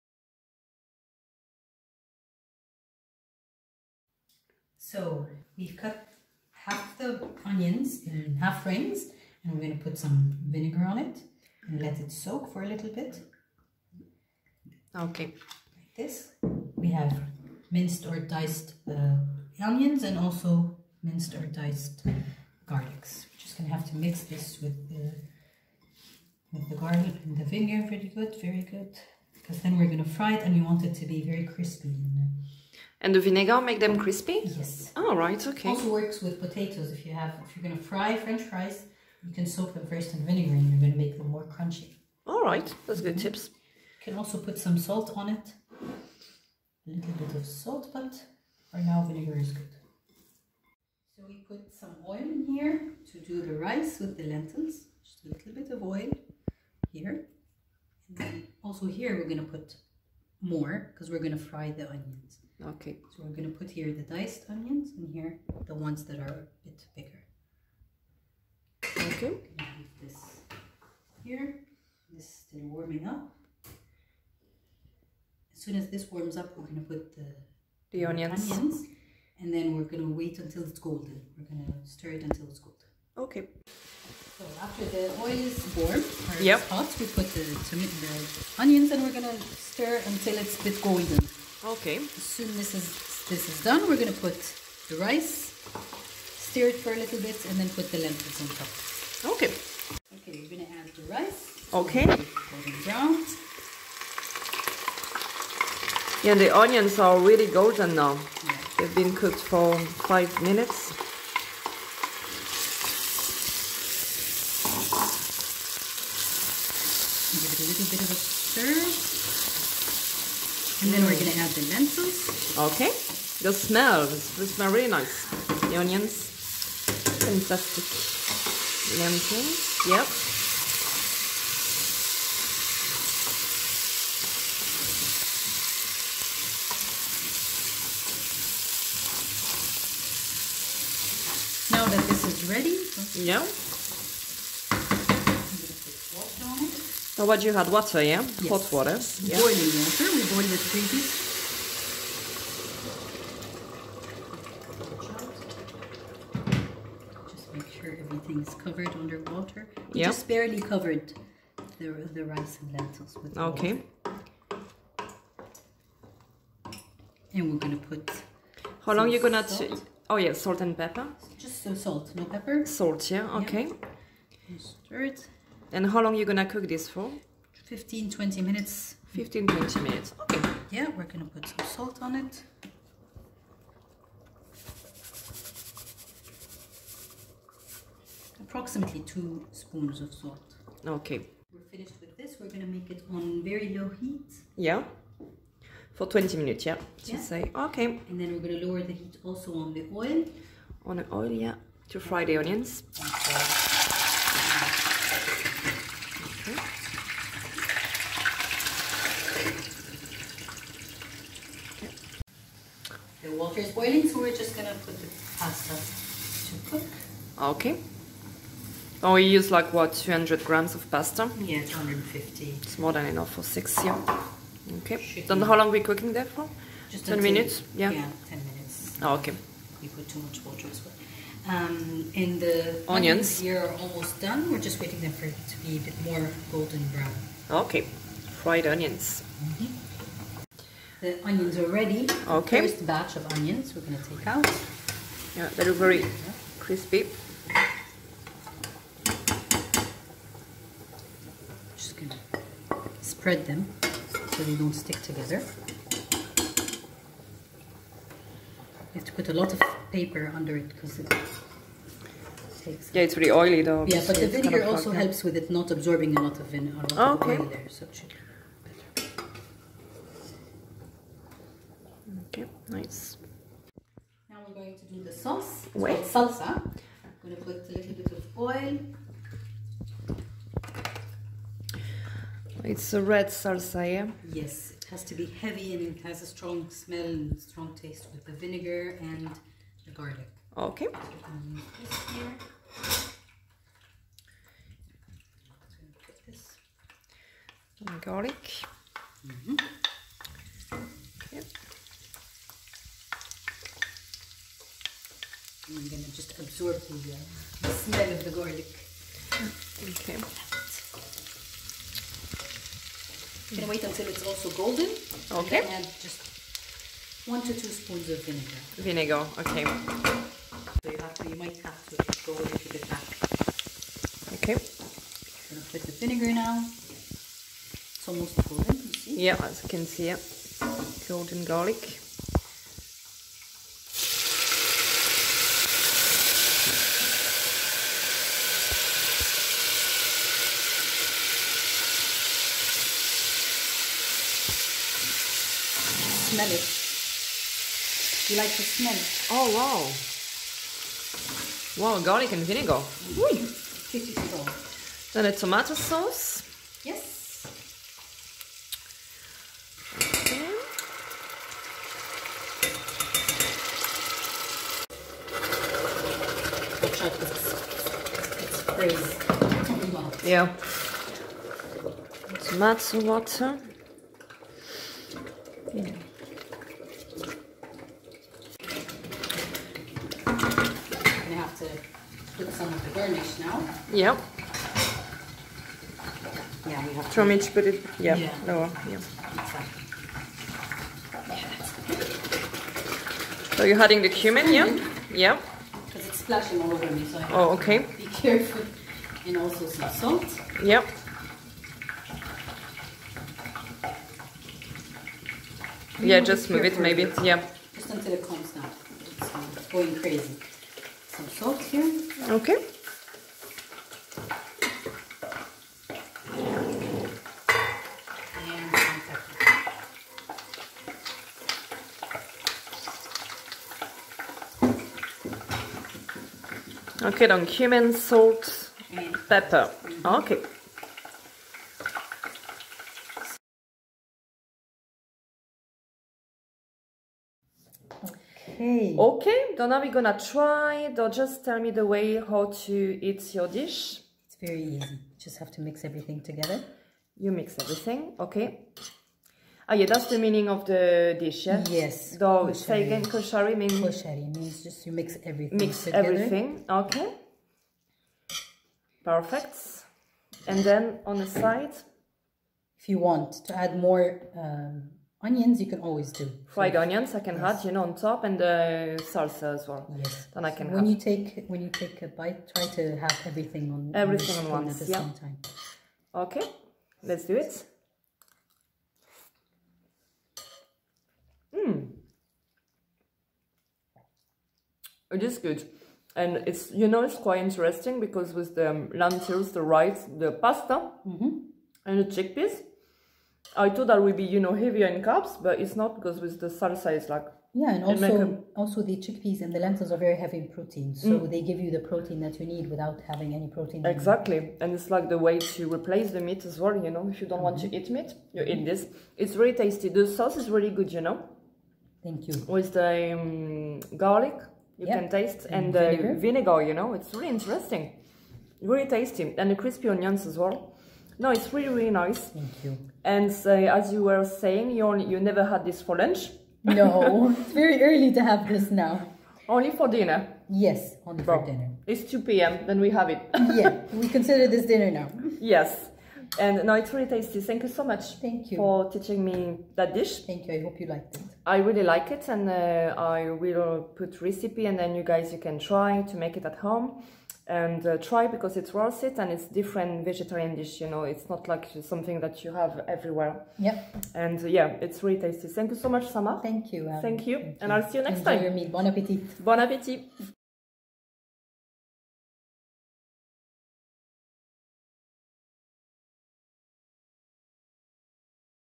Koshari made by Samar
Samar is from Cairo the capital of Egypte and we both met in Luxembourg for this special cooking day.
Samar cooked a traditionnal and famous recipe  from her country named Koshari.
It is a vegan and delicious dish.